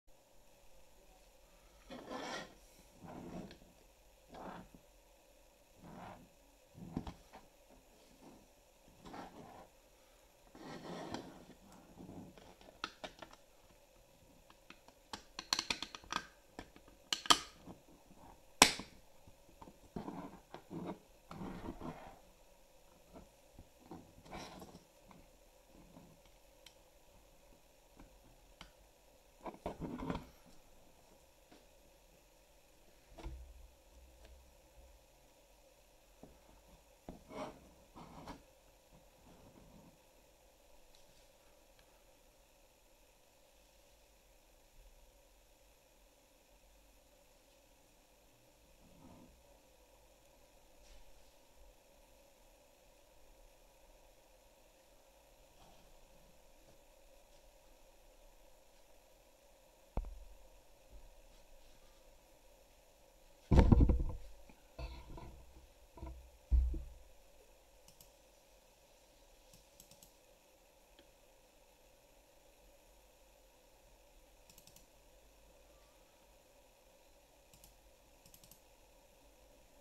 Assembly: Enclosure for pimoroni HyperPixel 4.0 Square Touch and Raspberry Pi 3 A+(Prusament)
Enclosure for pimoroni HyperPixel 4.0 Square Touch* (720x720 pixels (~254 PPI)) and Raspberry Pi 3 A+.

- direct mount of raspberry pi 3 A+ on HyperPixel
* it does *not* fit on HyperPixel 4.0 Square *Non-Touch*

Materials in Video are:
* Prusament PLA Vanilla White